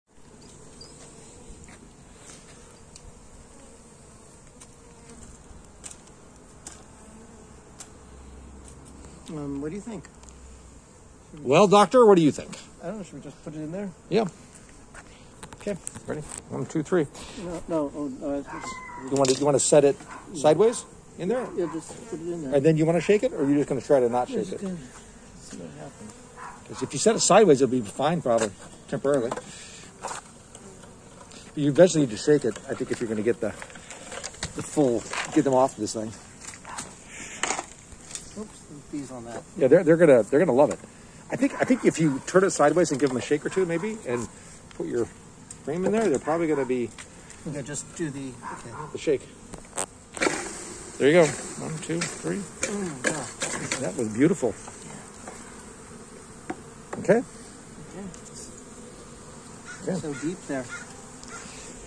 Leo Shakes Bees into a box edited
Leo captures a honeybee swarm from his own bees!